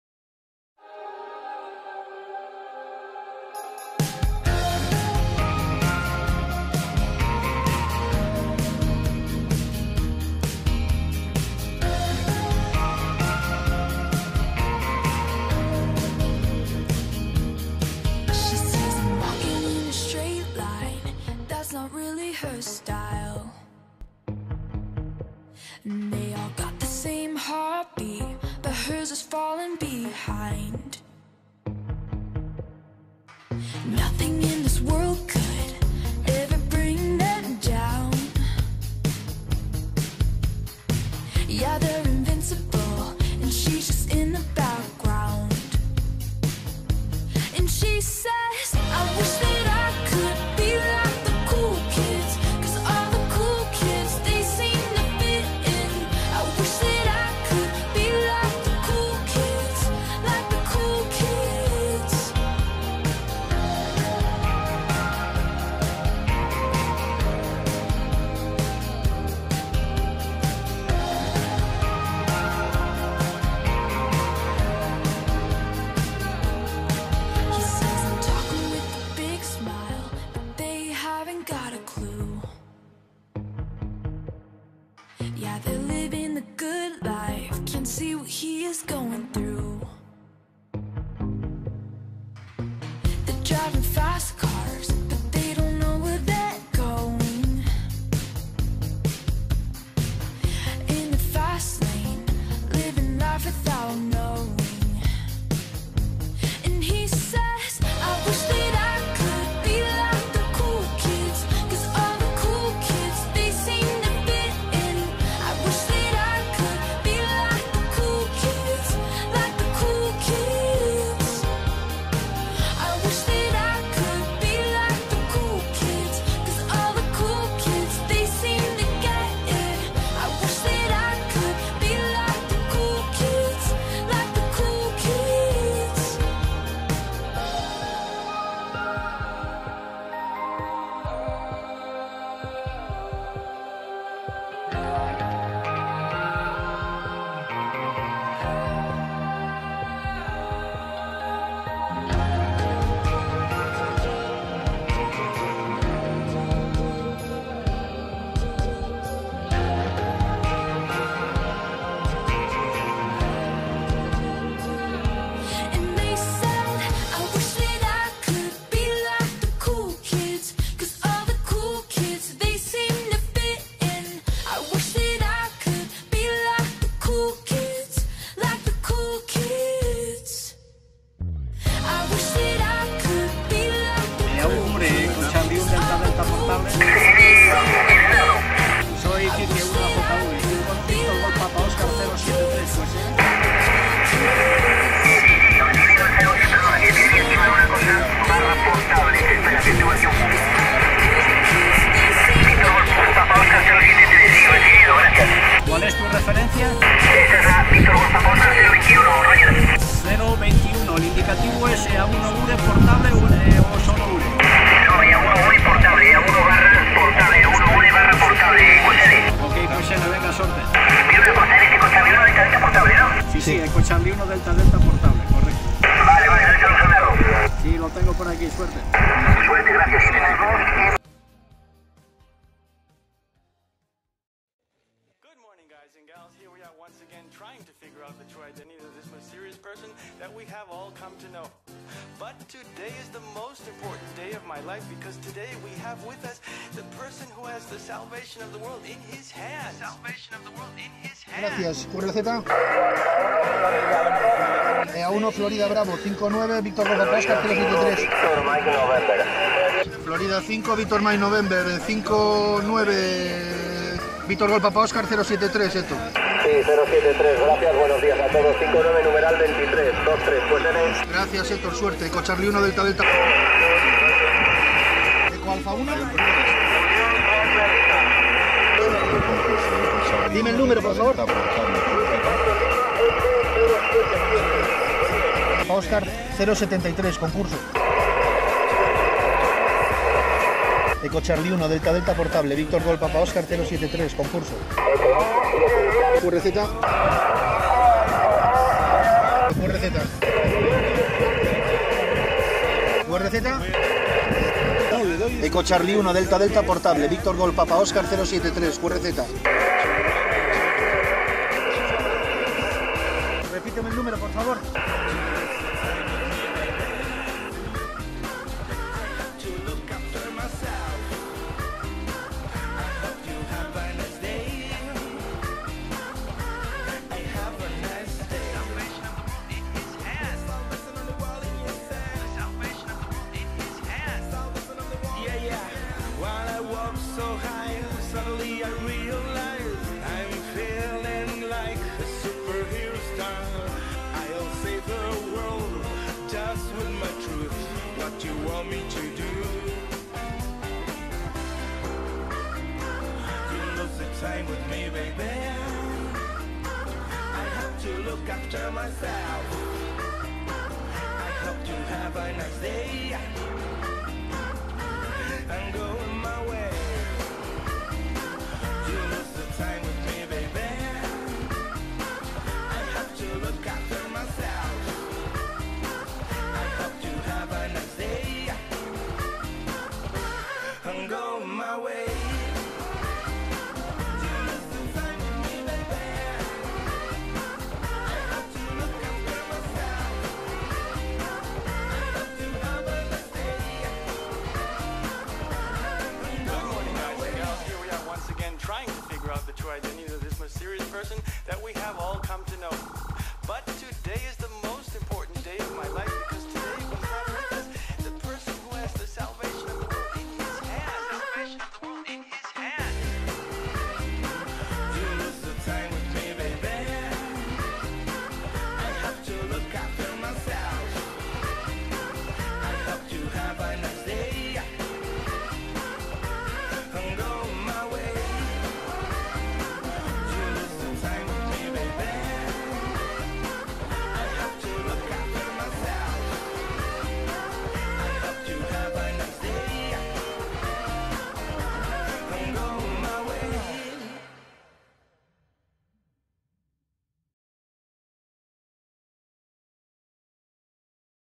EC1DD/P desde VGPO- 073
EC1DD/P
Operadores: EA1AJV ( Kike), EA1DGL ( Poldo), EA1IQM ( Berto) & EC1DD (Nardo).
Donuts Team Members desde las siguientes referencias:
VGPO-073 (Galleiro)
DME:36039 (O Porriño)
CG-053 (Comarca de Vigo)